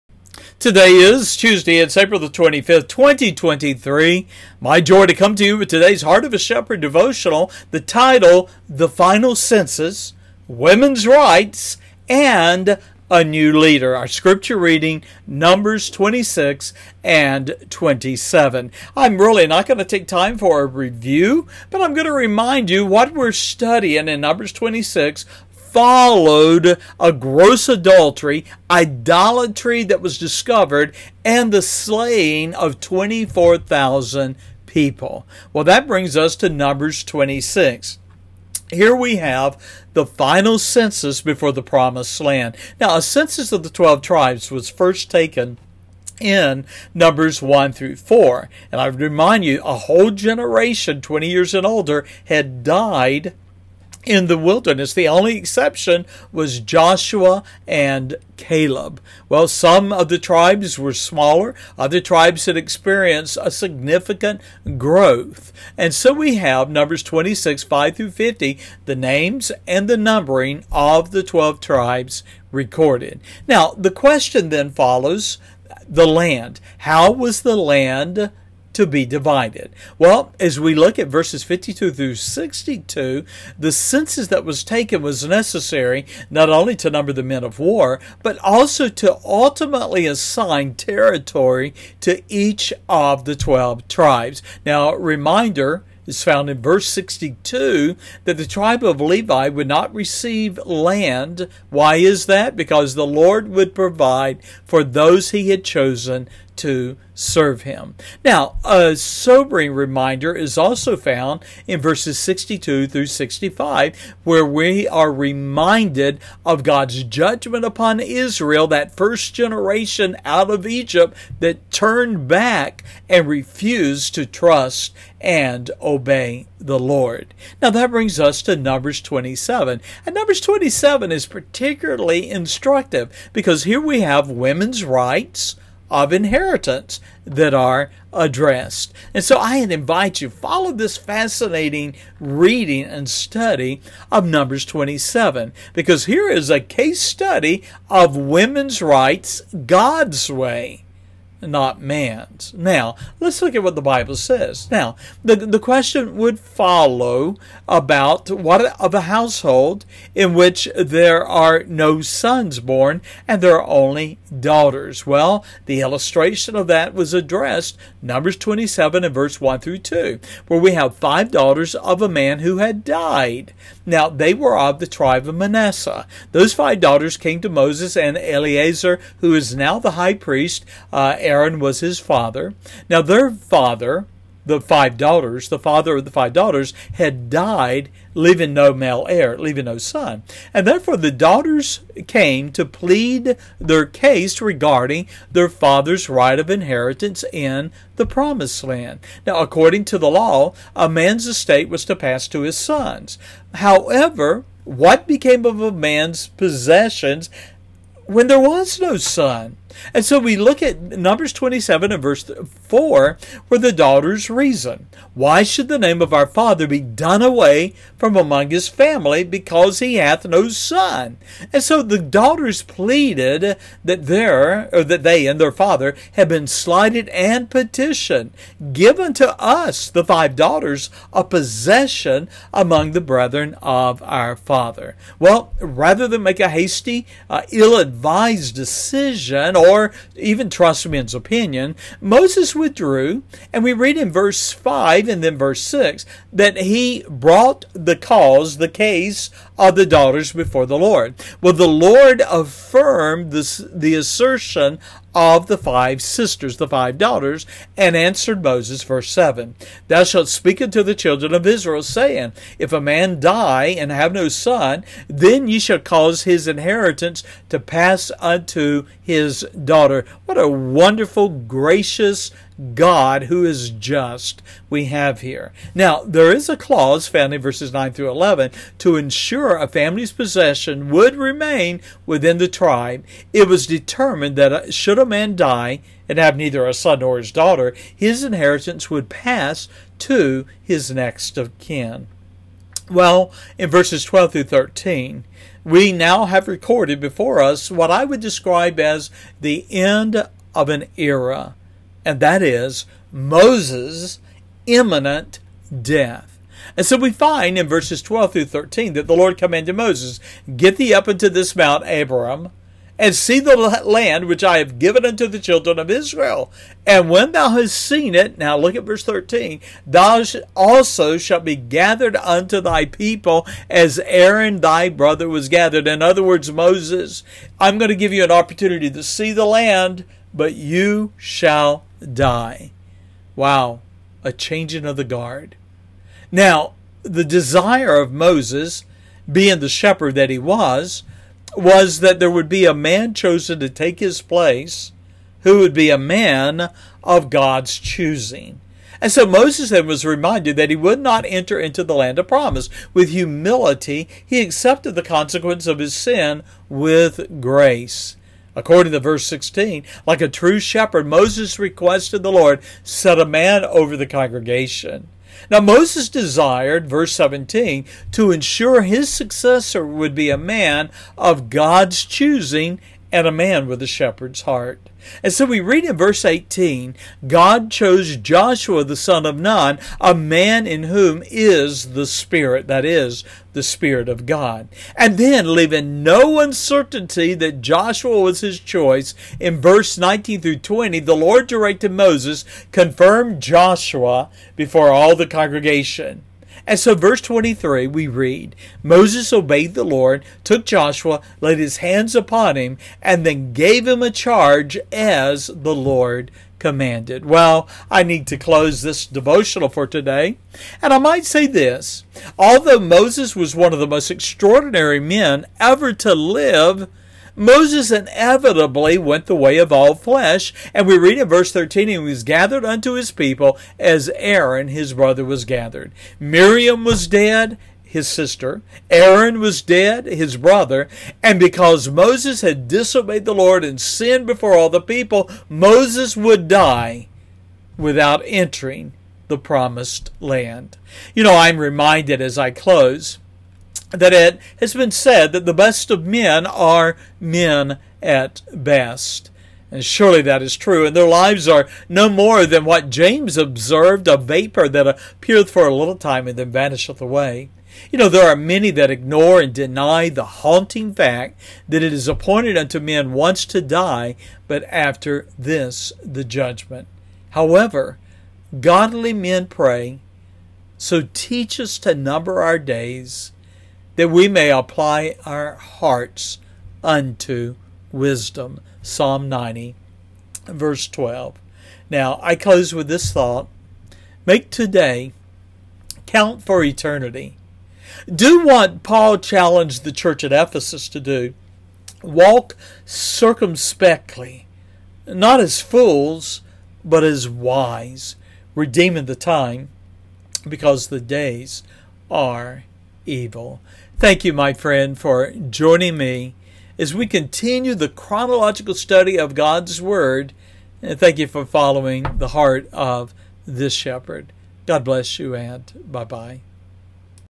The Final Census, Women’s Rights and a New Leader (Numbers 26-27) – A daily video devotion.
The LORD commanded Moses to take a final census before crossing the Jordan River. The census was necessary, for it became the basis for assigning each tribe territory in the Promised Land (26:52-56). In Numbers 27, we have the question of a woman’s rights of inheritance addressed. The LORD answered Moses: “Thou shalt speak unto the children of Israel, saying, If a man die, and have no son, then ye shall cause his inheritance to pass unto his daughter” (27:7).
Lastly, Numbers 27 records the end of an era and a leadership change as the LORD foretold Moses’ imminent death.